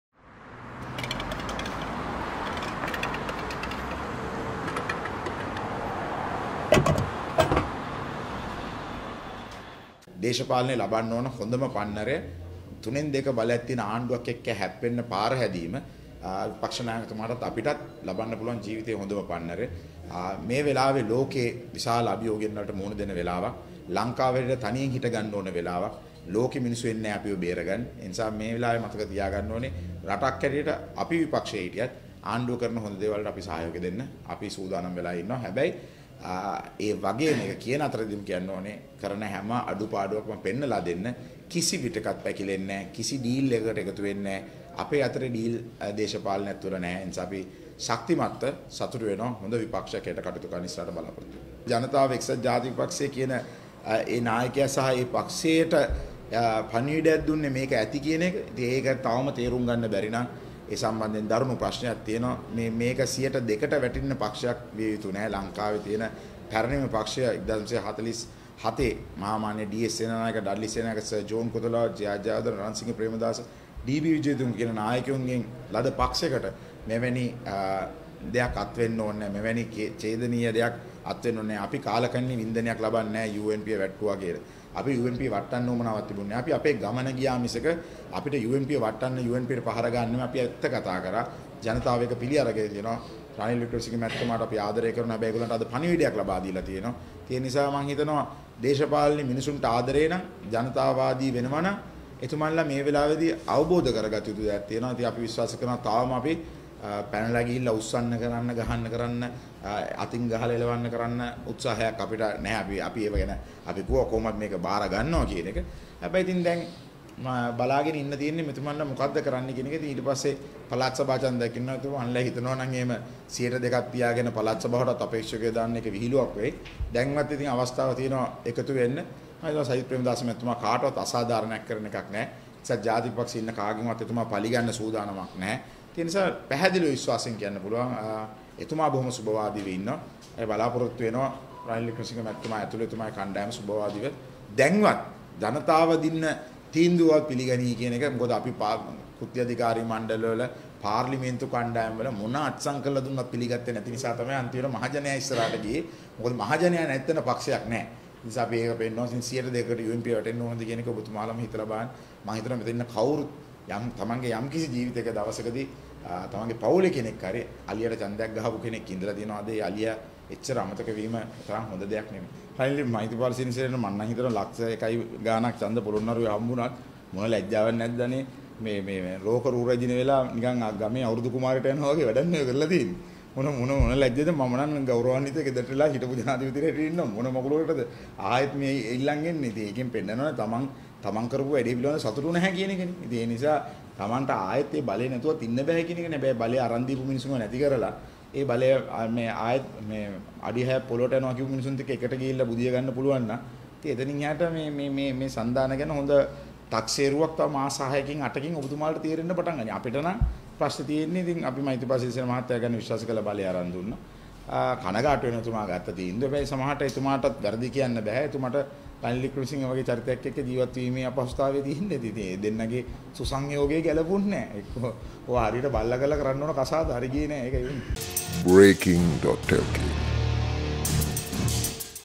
මෛත්‍රීපාල කලේ ලෝක රු රැජින වෙලා ඉදල ගමේ අවුරුදු කුමාරිට ගිහින් තියෙන්නේ | Breaking.lk
මොන ලැජ්ජා නැති වැඩක් ද මෛත්‍රීපාල කලේ ලෝක රු රැජින වෙලා ඉදල ගමේ අවුරුදු කුමාරිට ගිහින් තියෙන්නේ